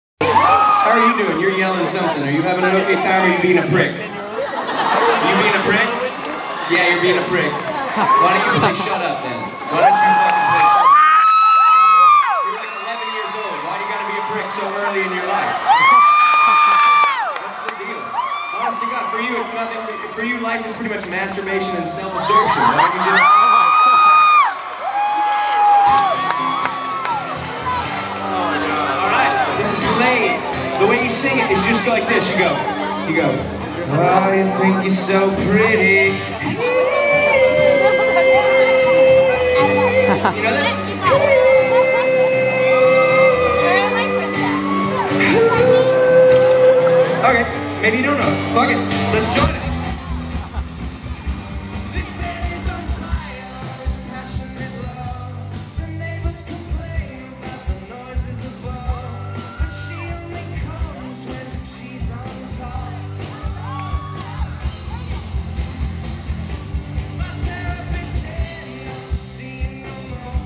the guy in the crowd is a prick
(i don't know what the name of the song is)
this was at Red White and BOOM 2009